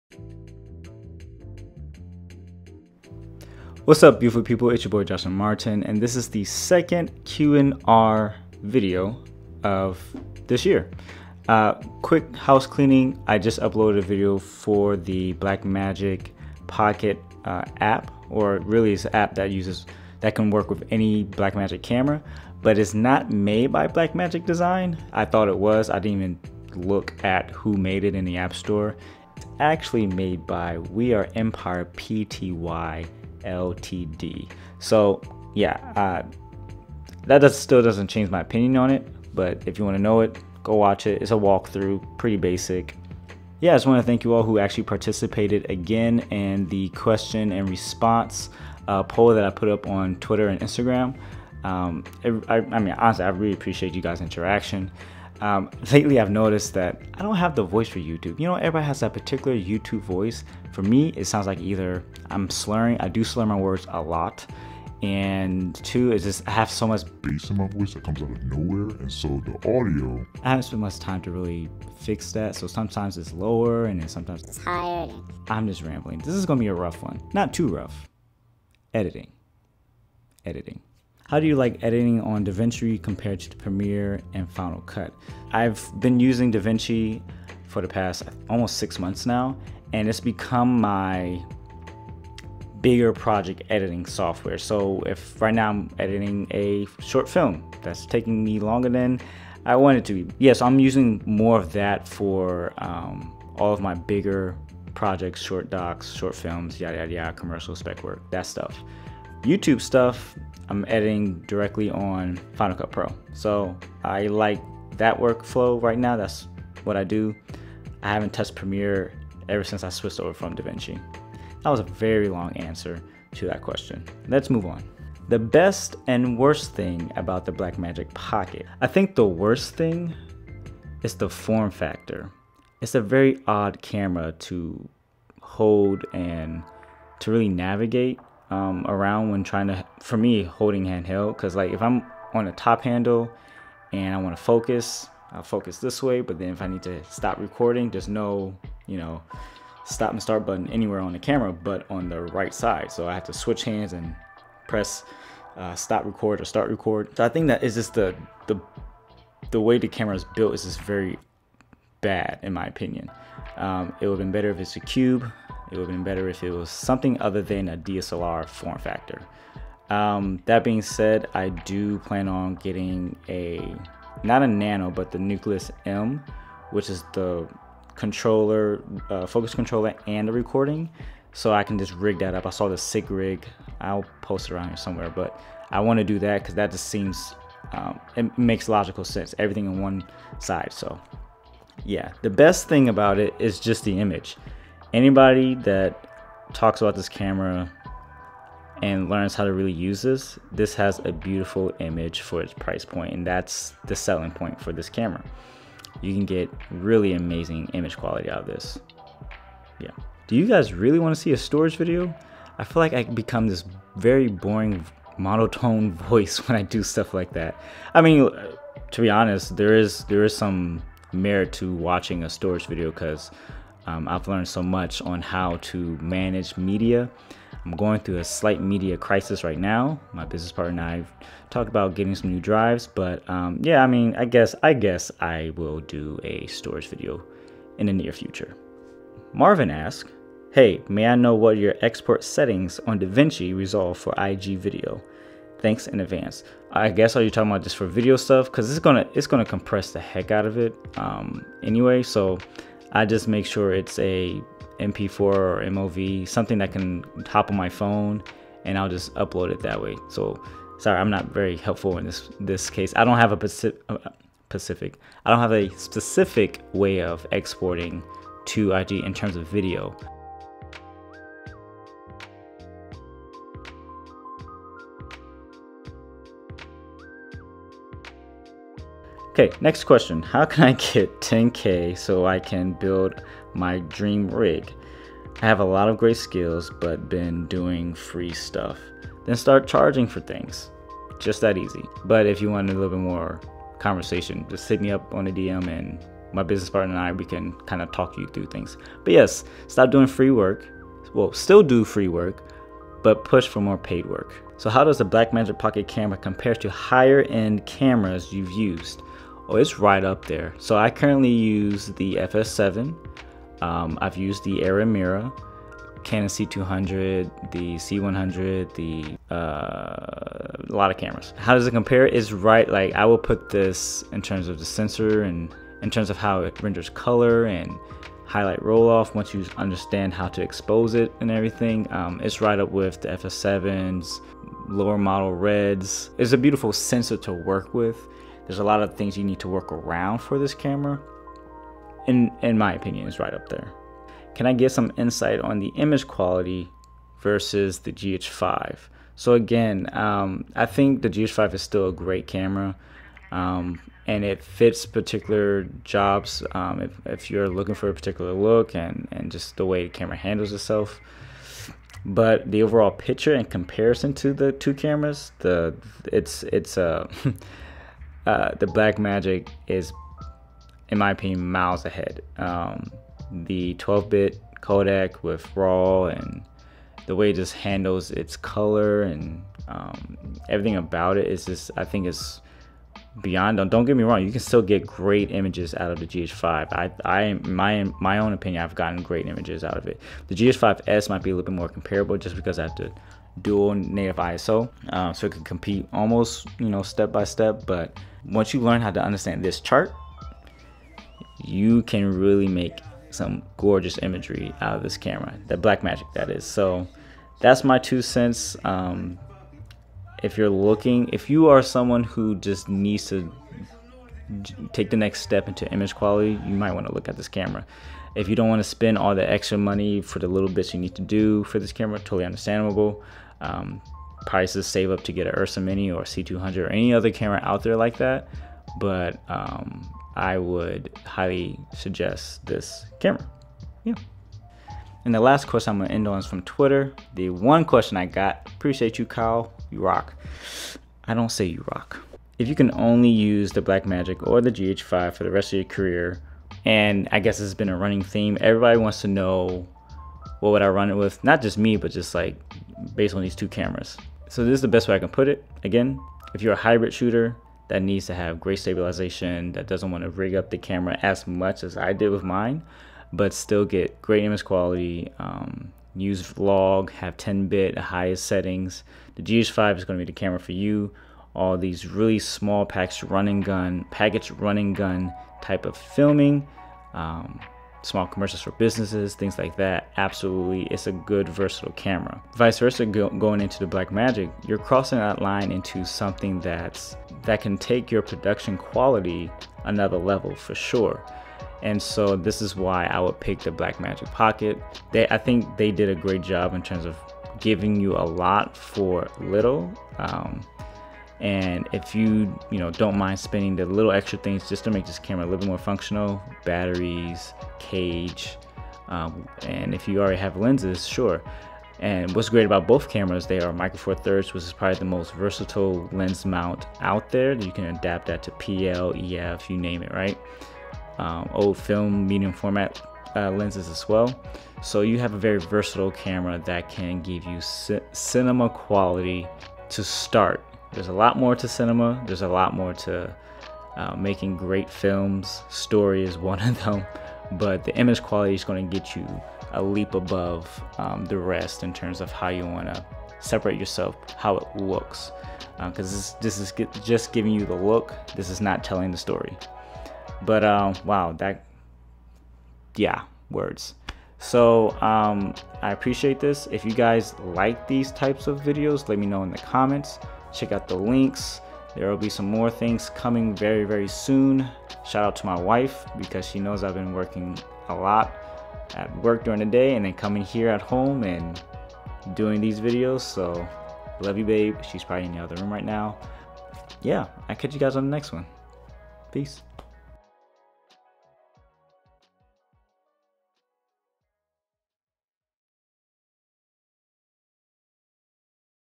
GH5 or BMPCC4K // AskJMS Q&R V2
Check out all of my gear on Kit Hey beautiful people! Here is another AskJMS upload. Thanks again to everyone who submitted questions. I plan on doing a video once a month, so keep the questions coming!